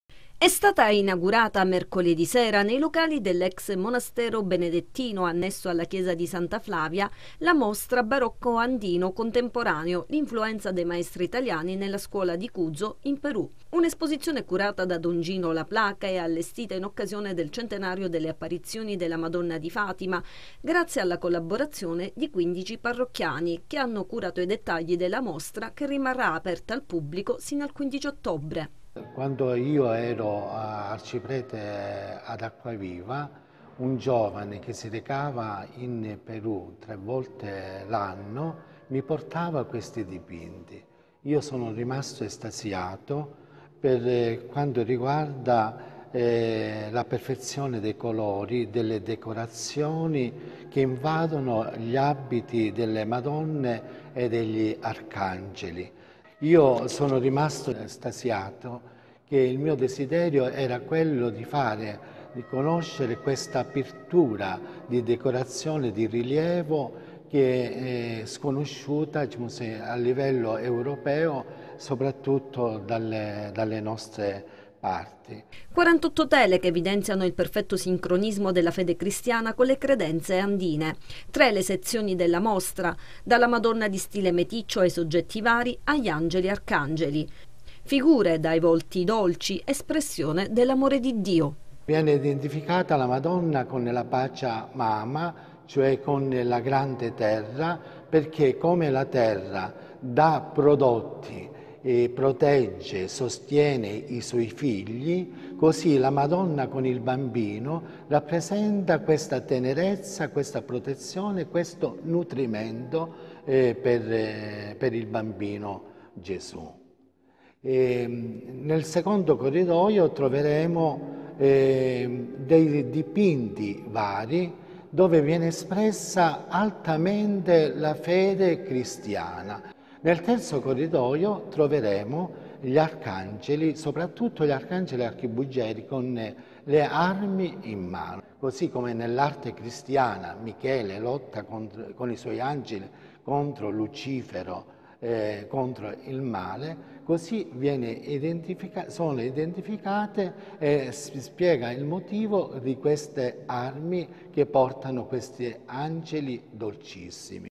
Mostra sul barocco andino
Una mostra sul barocco andino contemporaneo a Santa Flavia